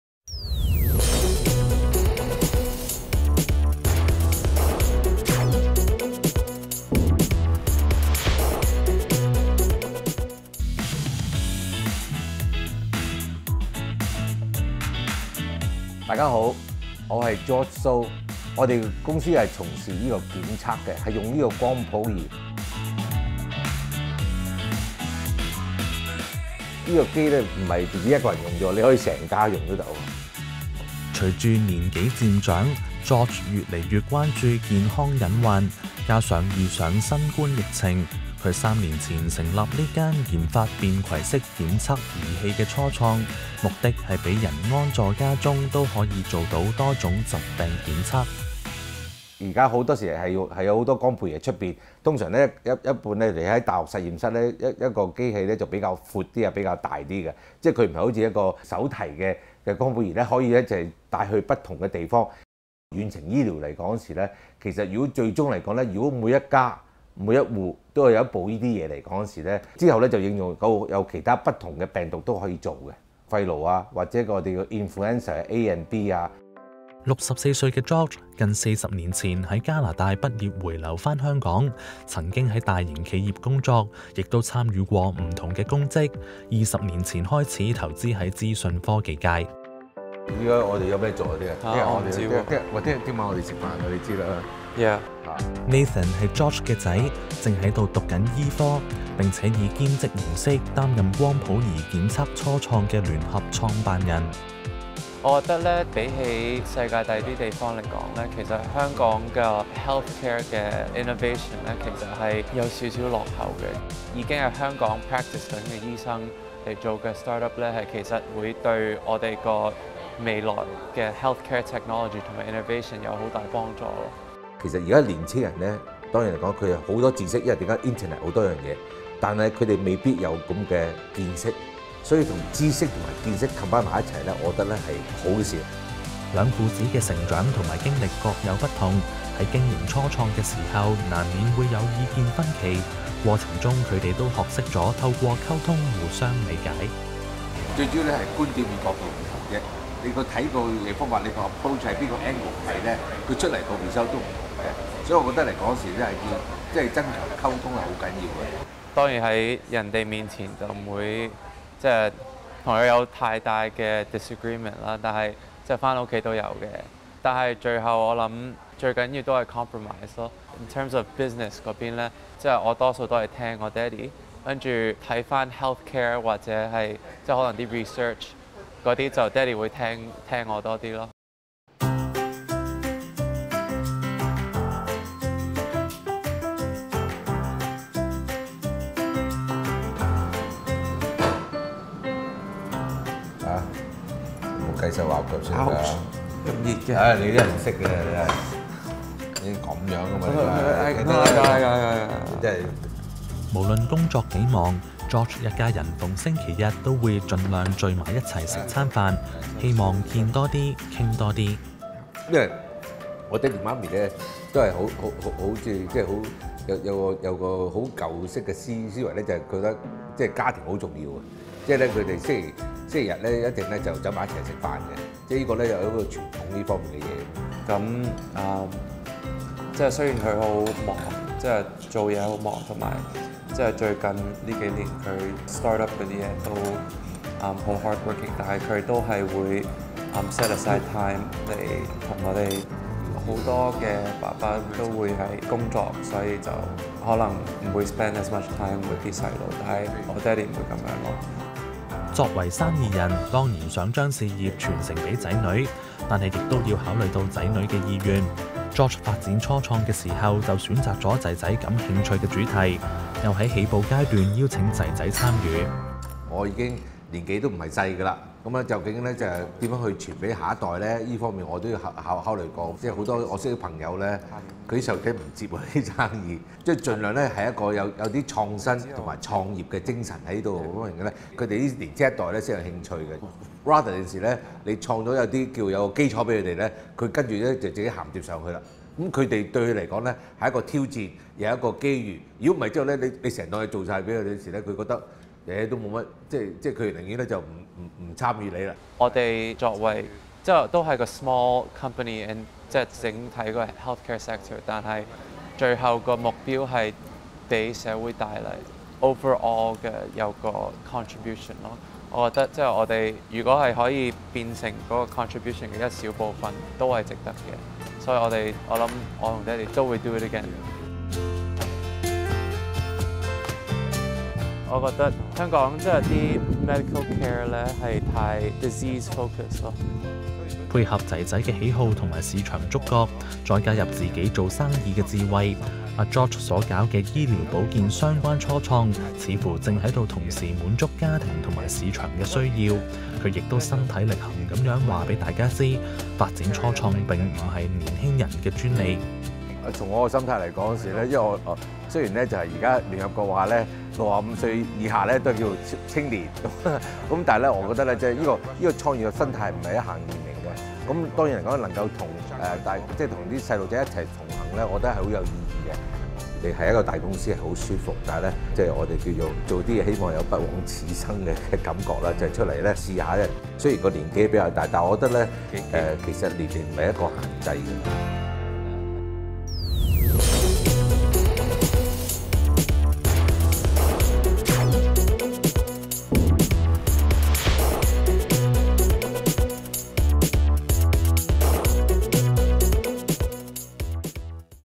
20230711【理財有方】初創之魂 無分年齡
有志發展初創豈會受年齡所限？有年過60歲的企業家看好疾病檢測商機，聯同正就讀醫科的兒子，創辦醫療保健初創，過程中有何啟發？兩父子有沒有遇到甚麼困難及機遇？